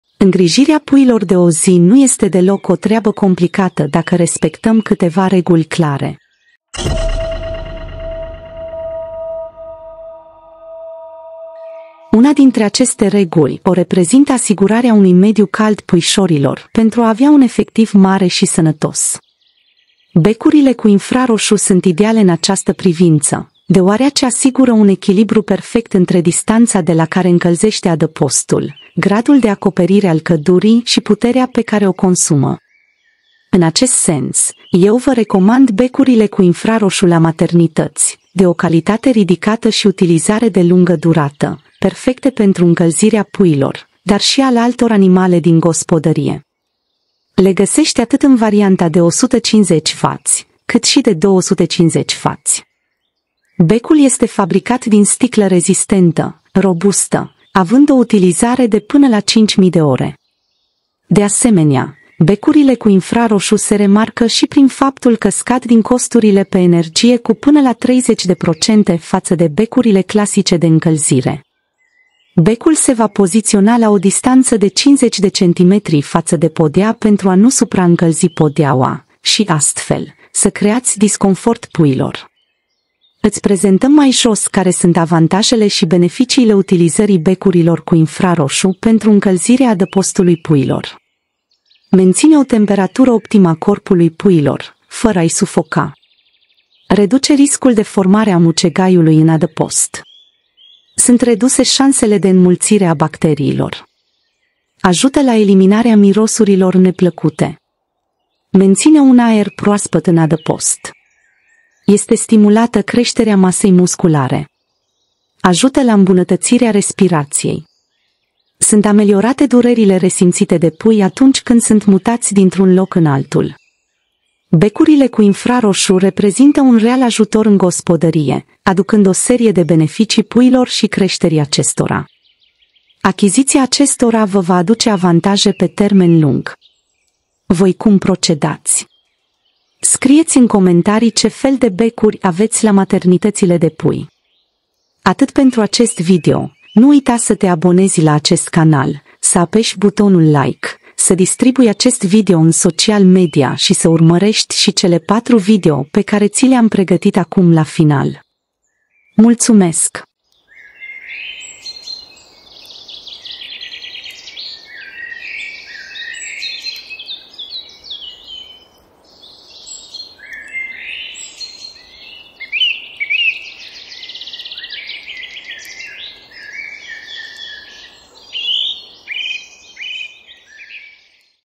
5 motive să folosești becul cu infraroșu la pui de găină
AnimaleSiPasariDePrinCurte. Doresc să creez o comunitate din care toată lumea să câștige, mai ales experiență în creșterea găinilor. Dar pentru asta te invit să te abonezi la acest canal in linkul de mai jos